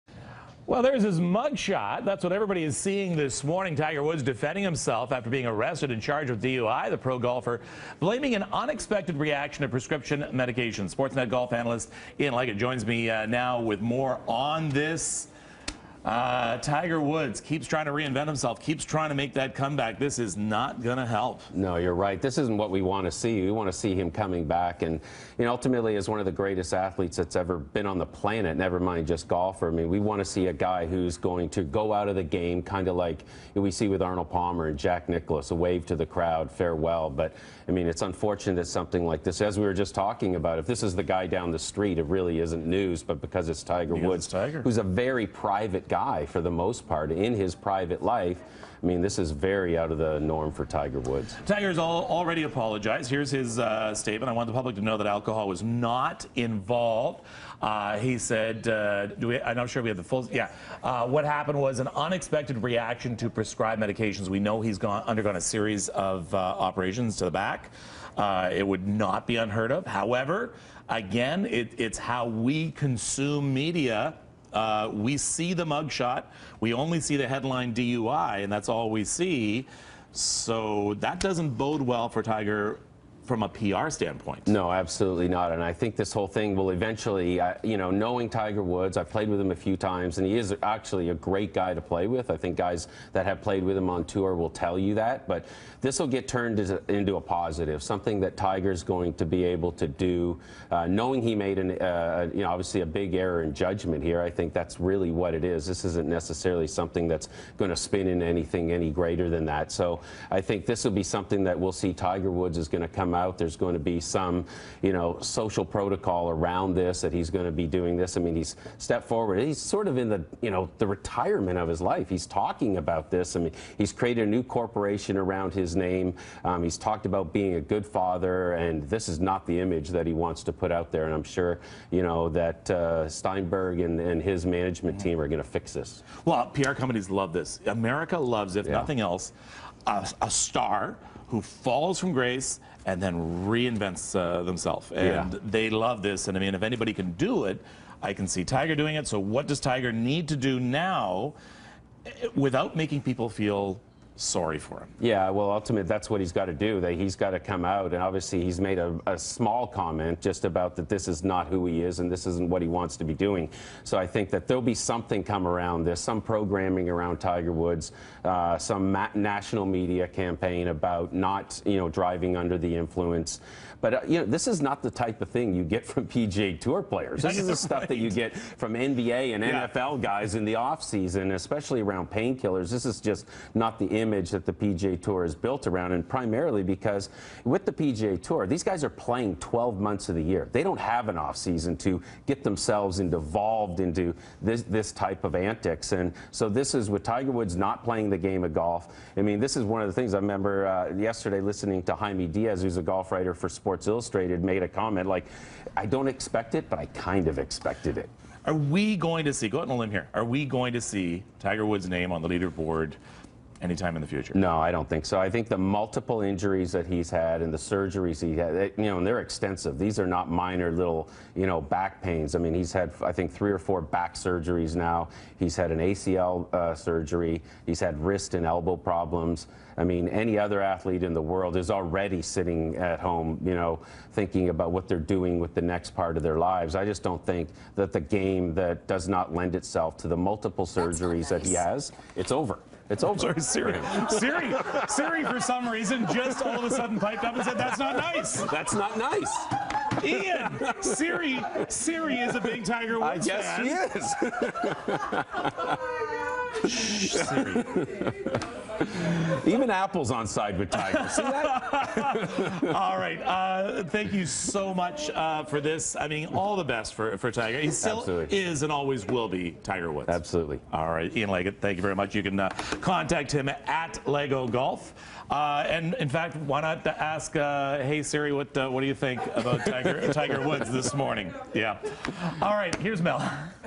Tiger Woods blames DUI on an unexpected reaction to prescription meds
Ian Leggatt, golf analyst, Sportsnet, talks Tiger Woods' DUI.